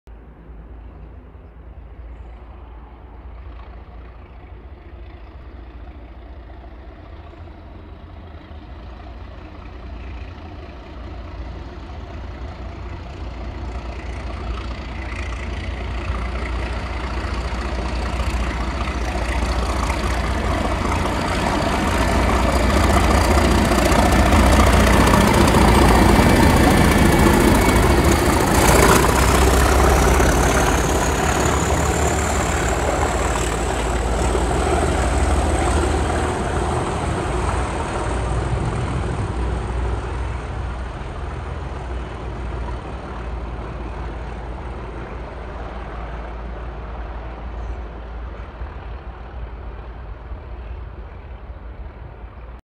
DRS Class 37423 with wagons passes Darlington on Doncaster to Motherwell 16/09/20
DRS Class 37423 with wagons passes Darlington on Doncaster to Motherwell

37423 returns back with some more wagons fresh from the wheel lathe and 37402 was left at York Parcel Sidings

16/09/20